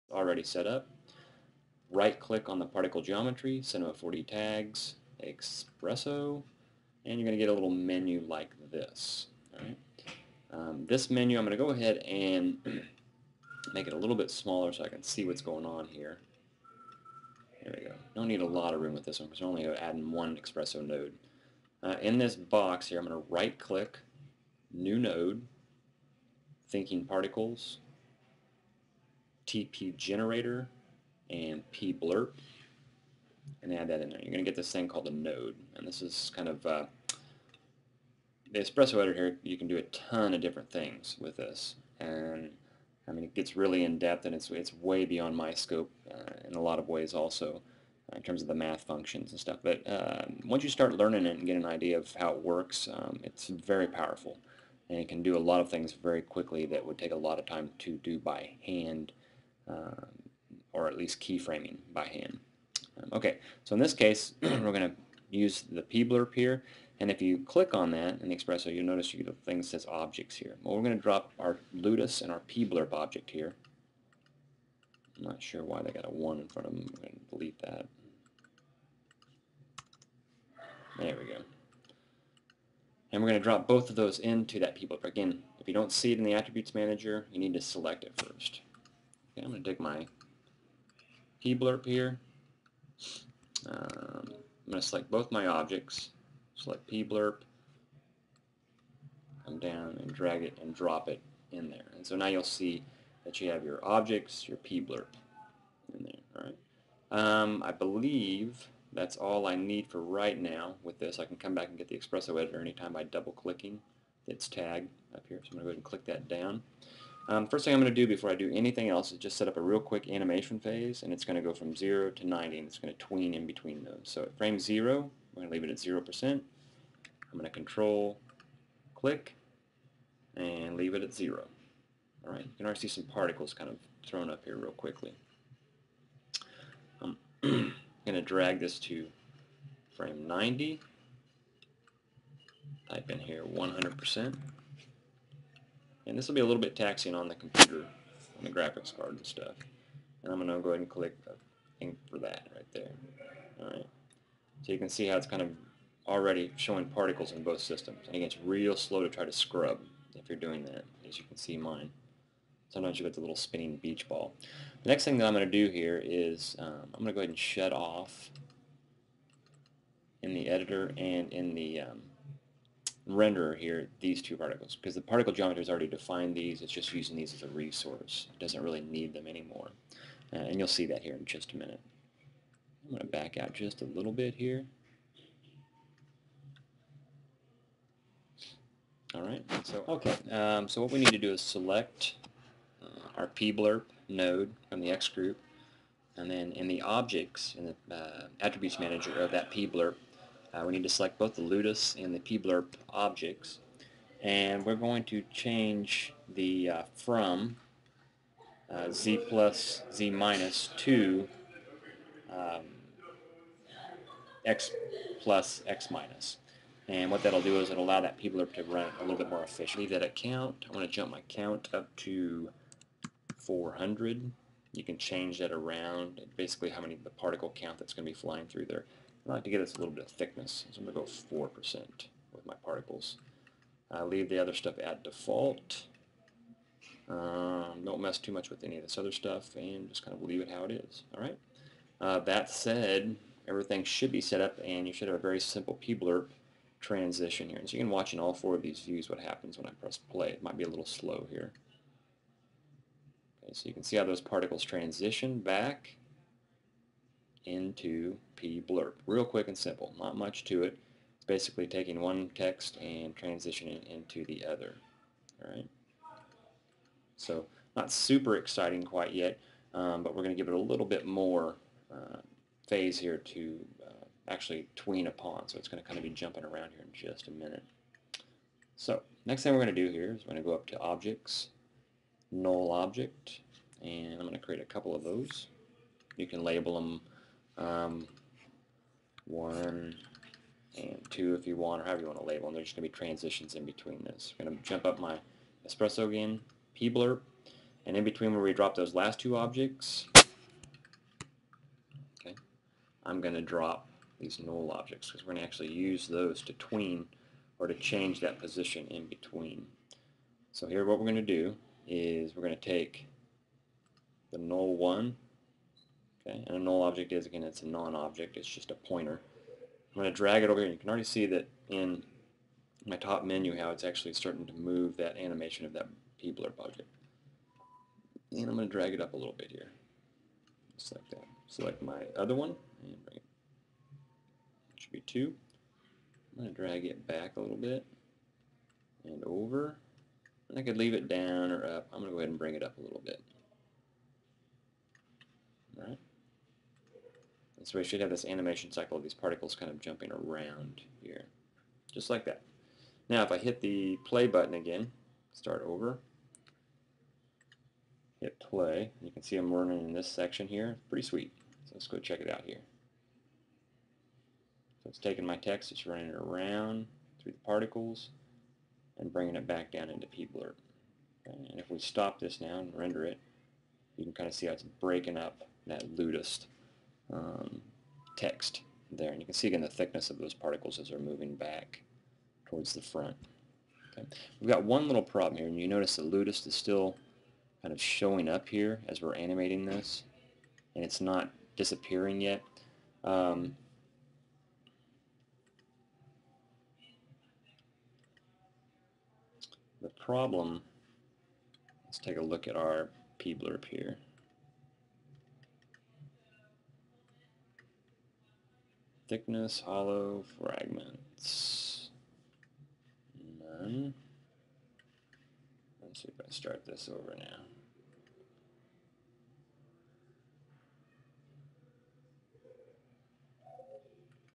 PBlurp Part 2
Part 2 of the PBlurp tutorial for TW High School 3D Computer Graphics class. Generously sampled from 696baby's tutorial